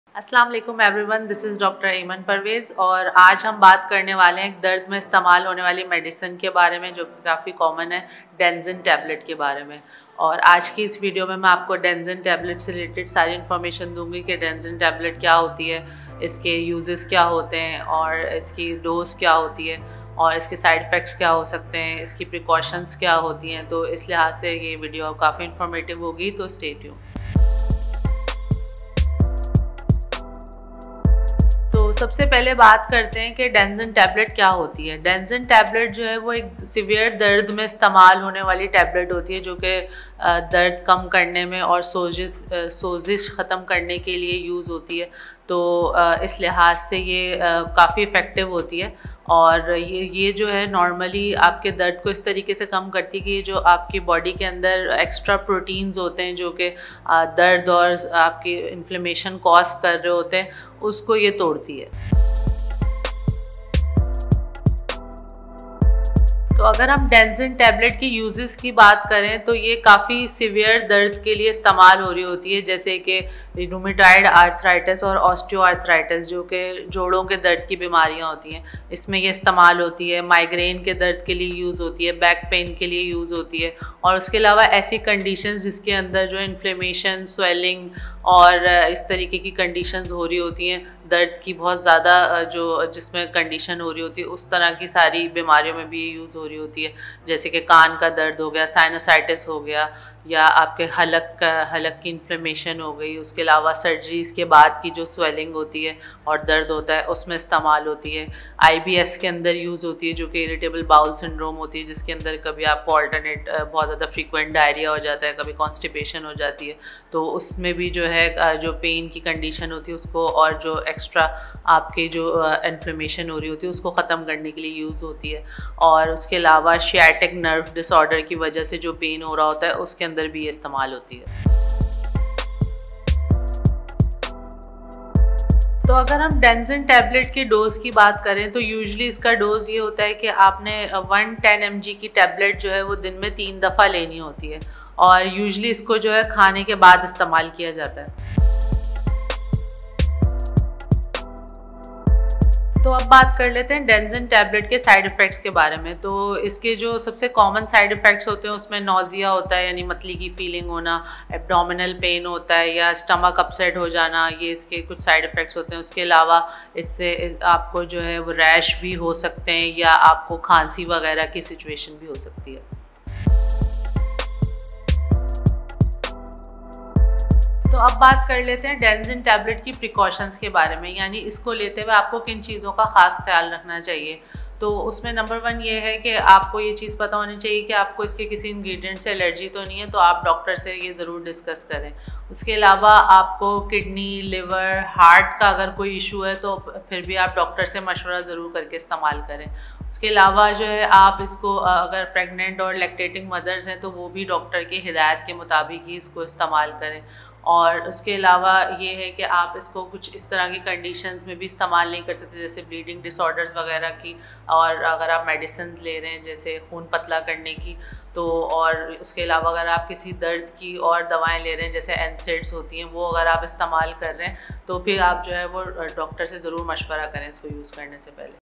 DANZEN TABLET - Uses, Side Effects, Dosage & Precautions - Dr. Review
00:00 Intro
00:27 what is DANZEN TABLET?
01:03 Uses of DANZEN TABLET?
02:12 Dosage of DANZEN TABLET?
02:28 Side effect of DANZEN TABLET?
02:53 Precaution of DANZEN TABLET?

What is DANZEN TABLET?
DANZEN Tablet is a tablet used for severe pain. It is very effective in reducing pain and reducing inflammation, and it reduces your pain by making extra proteins in your body, which reduce pain and inflammation cause it breaks it.

Uses of DANZEN TABLET?
DANZEN TABLET is used for severe pain, such as rheumatoid arthritis, and osteoarthritis, which are joint pain diseases. It is used for migraine pain, back pain, and conditions such as inflammation, soiling, and such conditions. It is used in all such diseases as the conditions of pain are increasing It is used in ear pain, Sinusitis, and Throat inflammation, besides swelling and pain after surgery. IBS stands for Irritable Bowel Syndrome, in which there is a lot of alternate frequent diarrhea, sometimes there is constipation, it is a condition of pain, and it is used to eliminate the extra inflammation that is happening. And It is also used internally for pain caused by the sciatic nerve disorder.

Dosage of DANZEN TABLET?
DANZEN Tablets The usual dosage is 1 tablet of 10 mg three times a day and is usually taken after meals.

Side Effect of DANZEN TABLET?
There are the most common side effects of DANZEN Tablet:
Abdomen pain,
Nausea,
Upset stomach,
These are some of its side effects and you may also become nauseous or have a cough situation.

Precautions of DANZEN TABLET?
Before using DANZEN tablet, you should make sure that you are not allergic to any of its ingredients, and if you have kidney, liver, or heart problems, consult your doctor. In addition, if you are a pregnant and lactating mother, then use it as per the doctor's instructions. In addition, you cannot use it in some conditions such as bleeding disorders, etc. If you are taking a blood-thinning medicine, And if you are taking any other pain medication like anset, then you must consult your doctor before using it.